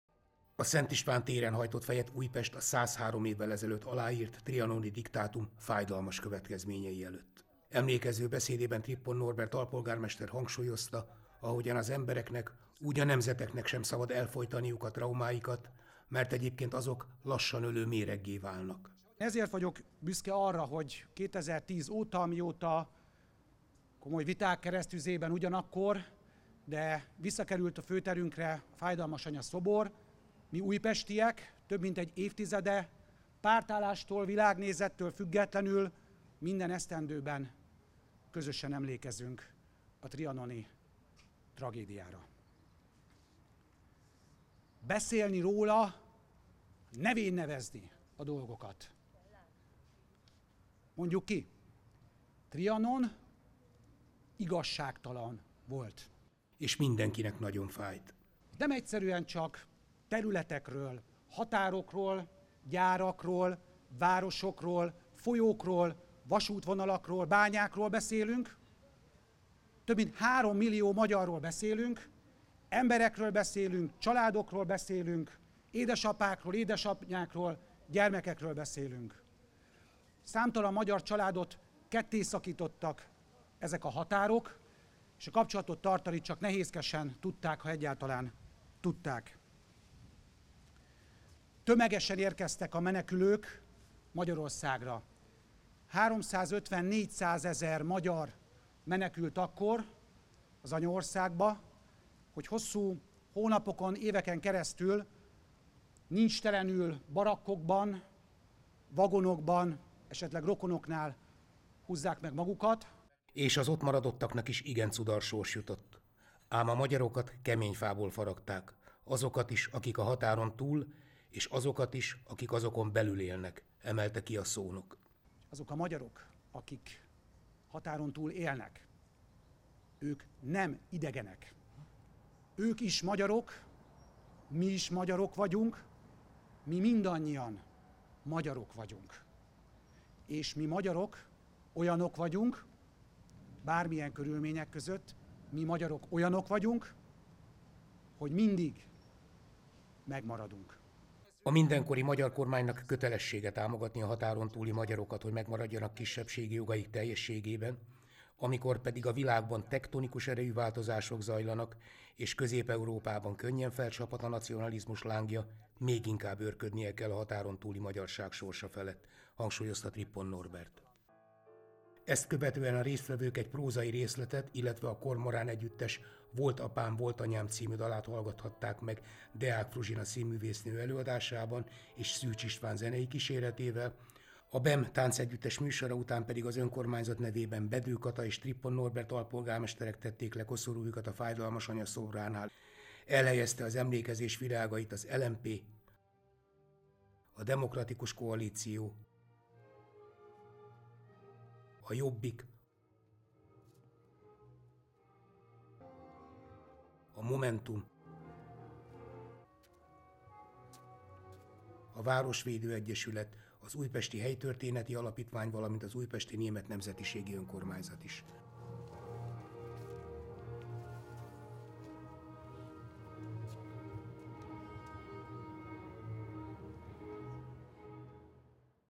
Körkép - Az országot 103 éve megcsonkító trianoni békediktátumra emlékezett Újpest
A főtéren álló Fájdalmas Anya-szobornál emlékezett Újpest az országot 103 éve megcsonkító trianoni békediktátum tragikus következményeire. Ezek máig súlyos és felelősségteljes munkát rónak a mindenkori kormányra, amelynek kötelessége támogatni a határon túli magyarokat, hogy megmaradhassanak kisebbségi jogaik teljességében – hangsúlyozta beszédében Trippon Norbert alpolgármester.